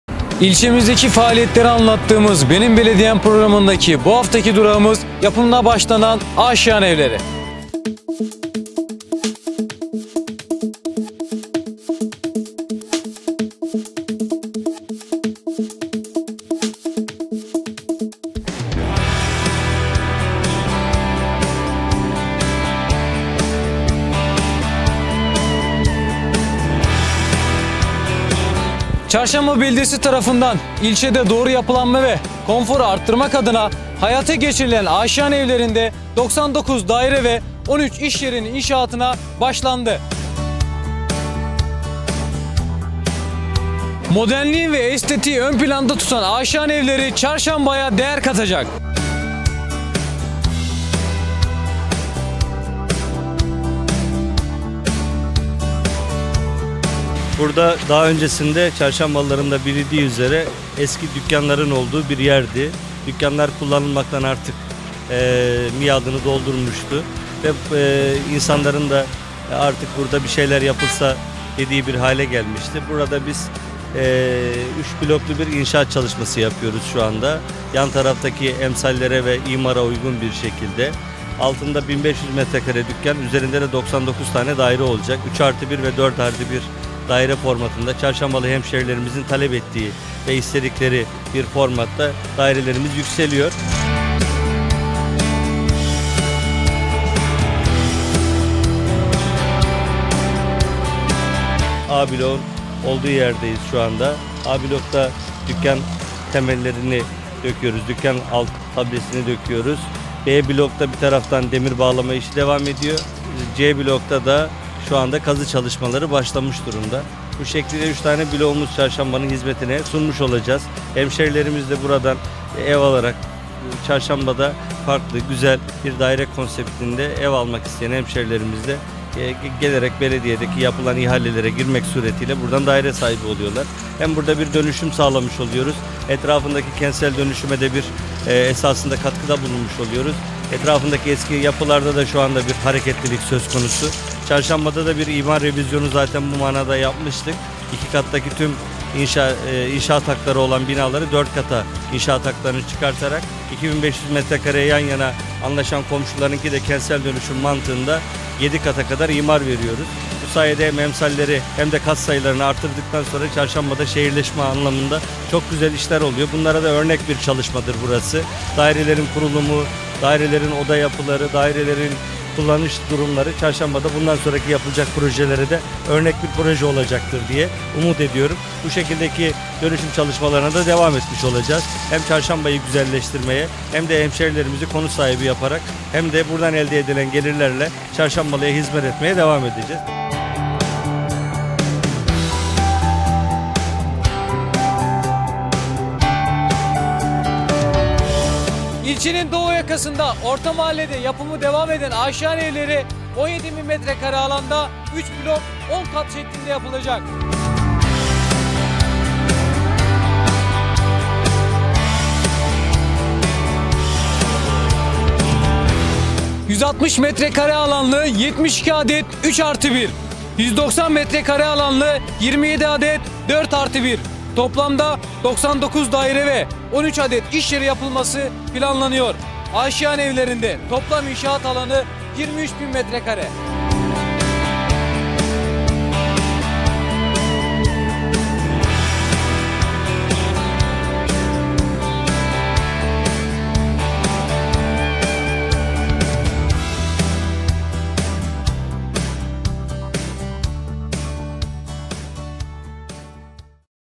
BENİM BELEDİYEM: AŞİYAN EVLERİ
📌Aşiyan Evleri inşaatımız yükseliyor..

Çarşamba’mız için çalışmaya, ilçemize hizmet etmeye devam ediyoruz.

Aşiyan Evleri'miz ile hem Çarşamba’yı güzelleştiriyor hem de kentsel dönüşüme katkı sağlıyoruz..